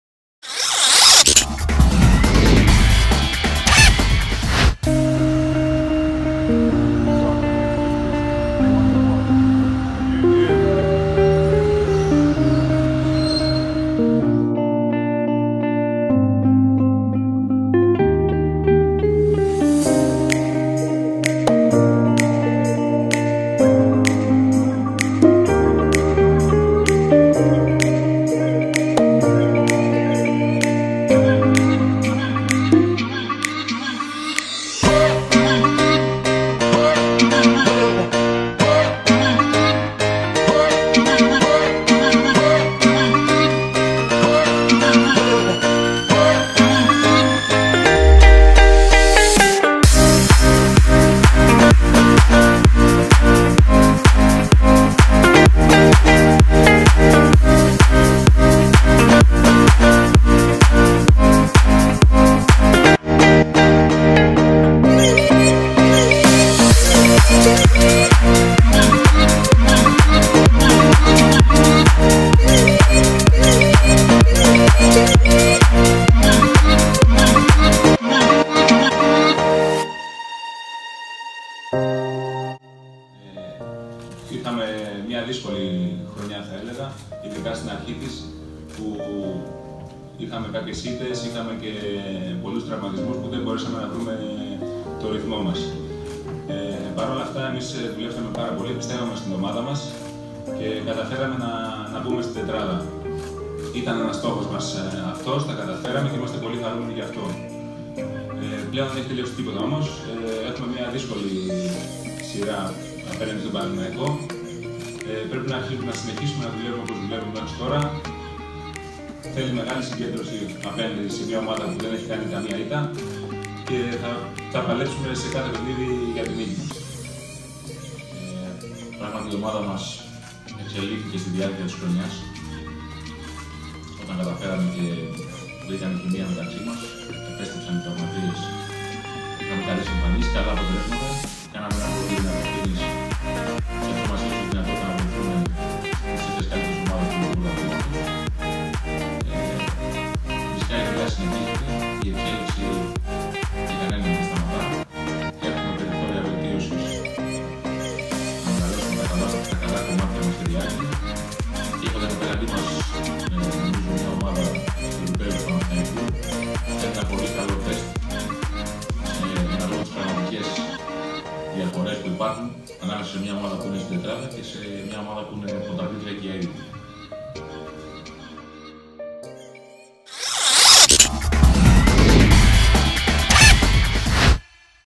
Ο ΠΑΟΚ στην Αθήνα και στα ημιτελικά
Η αναχώρηση της αποστολής του ΠΑΟΚ για την Αθήνα, οι ξεχωριστές στιγμές στο μουσείο της Ακρόπολης με το τρόπαιο και η συνέντευξη Τύπου για τα ημιτελικά των playoffs